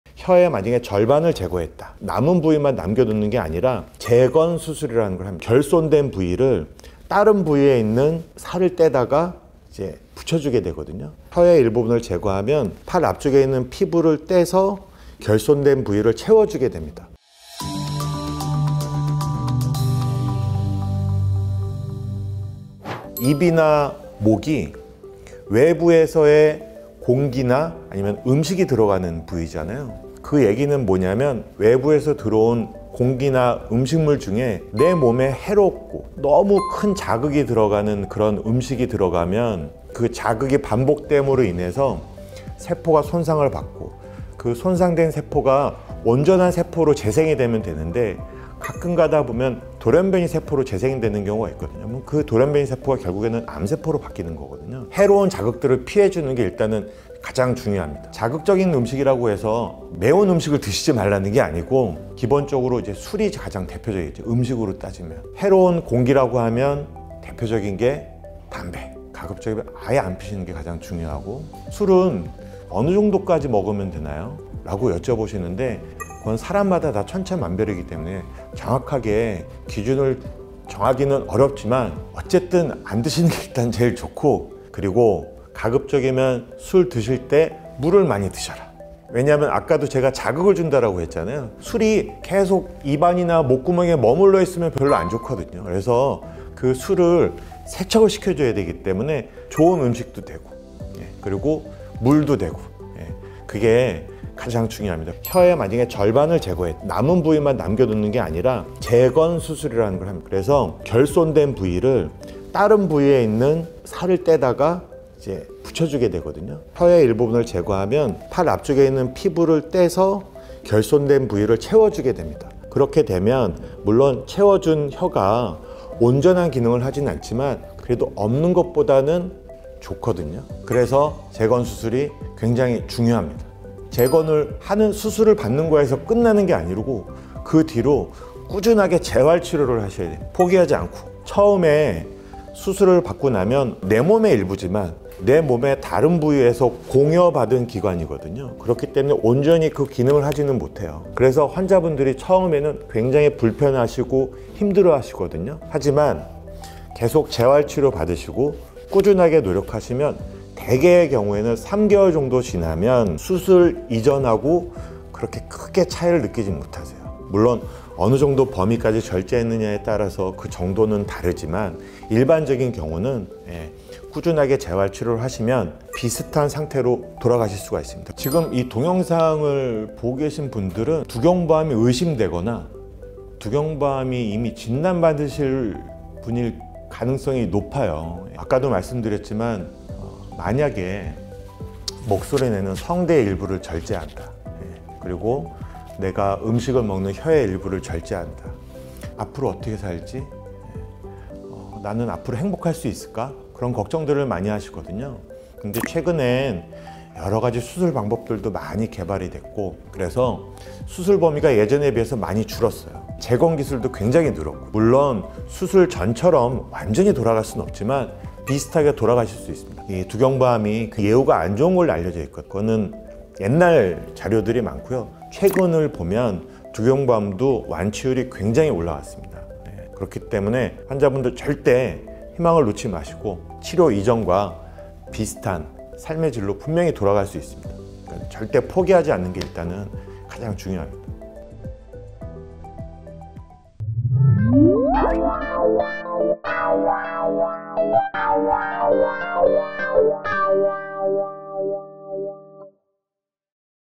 [ENG] 모양은 이전과 비슷하지만 기능도 다시 돌아올 수 있을까? | 두경부암
성대나 혀의 일부를 절제 및 재건 수술까지 마친 환자들이 가장 걱정하는 것은
나의 몸이 예전과 같은 기능을 할 수 있을까라고 생각됩니다

재활훈련을 거친다면 회복하는데 얼마나 걸리는지
완치율은 어떤지에 대하여 궁금한 점이 많으실텐데요

오경호교수님의 흥미로운 설명과 함께
두경부암을 낱낱이 파헤쳐 봅시다👊👊!

00:00 오프닝
00:23 두경부암의 예방법은?
01:44 두경부암 수술 후에는?
02:13 재건 수술 후 관리는?
02:57 환자들에게 당부하고 싶은 말

👨‍⚕️고려대학교 안산병원 이비인후・두경부외과 오경호 교수